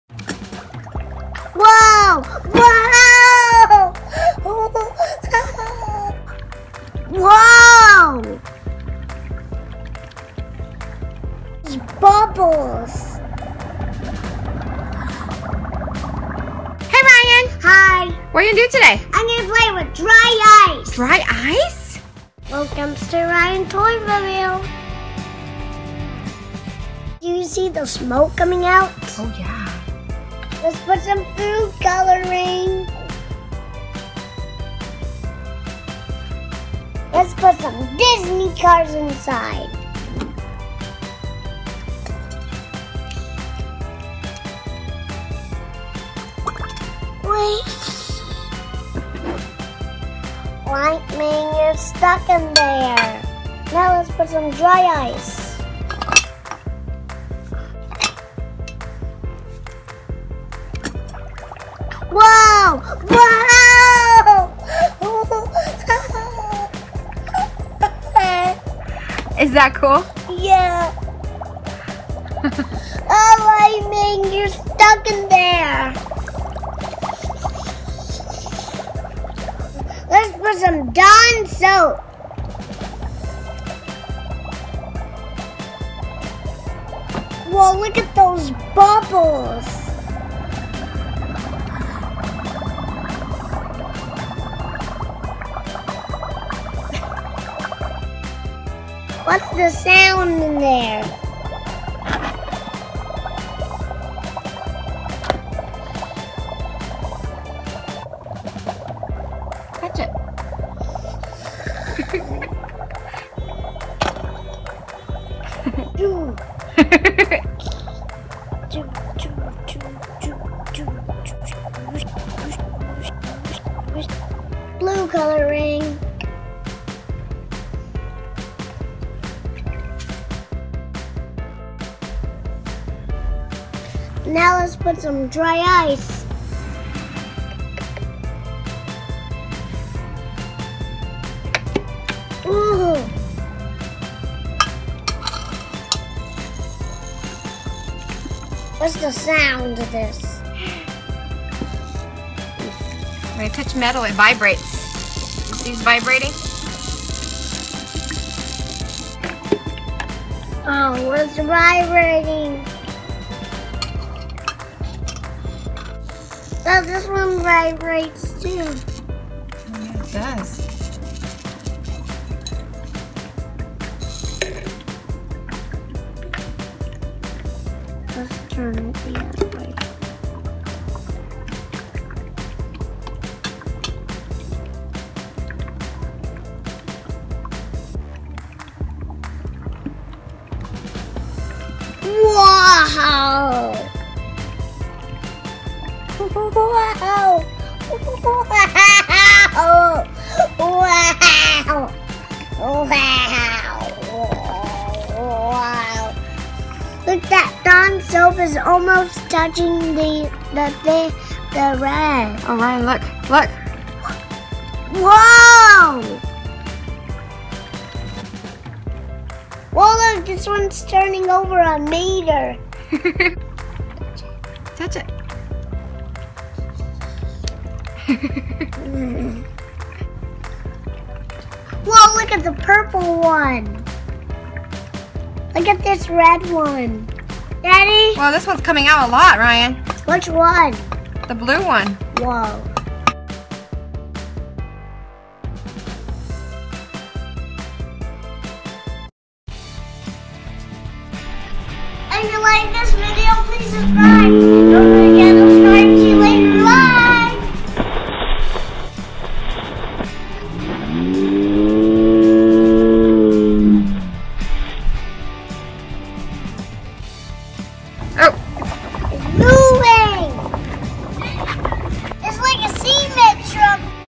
DRY ICE EXPERIMENT Easy science experiments for Kids with Disney Toys
Ryan from Ryan ToysReview is doing another amazing easy science experiments for kids to do at home with the help of car toys like Disney Pixar Cars Lightning McQueen!

NOTE: Parent Help is needed and do not handle the dry ice with hand!!!
All you need is Dry ice, water, and dish washing soap! When you mix the dry ice with water, you can see awesome smoke and fog coming out! You can also create bubbles when you add some dishwashing soap into the dry ice and water mix! This is such a fun activities for kids and parents for an amazing family fun experience together!